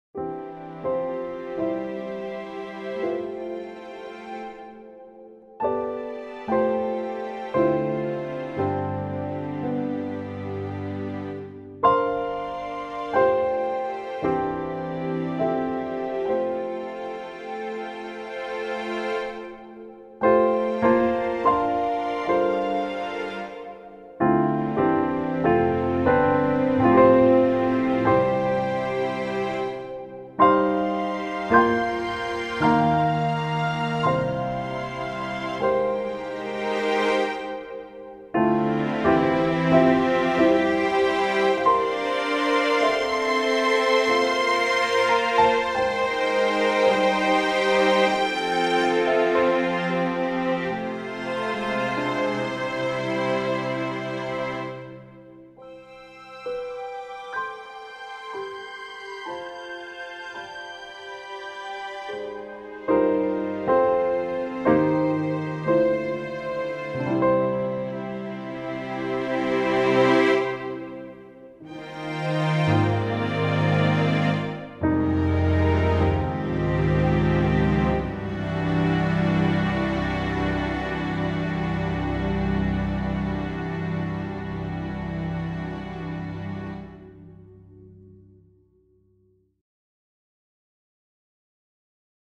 Spitfire Audio Originals Intimate Strings (demo) | Dave Hiltebrand
Here is a sample of the new Spitfire Audio Intimate Strings. This Long Half Section patch layered with their LABS Glass 1 Piano Pad under a grand piano make an incredibly inspiring sound. This is a sound my younger self would have dreamed to have. I especially love the dynamic range you hear between 0:40 and 1:08. So many emotionally rich sounds at your fingertips. Love it.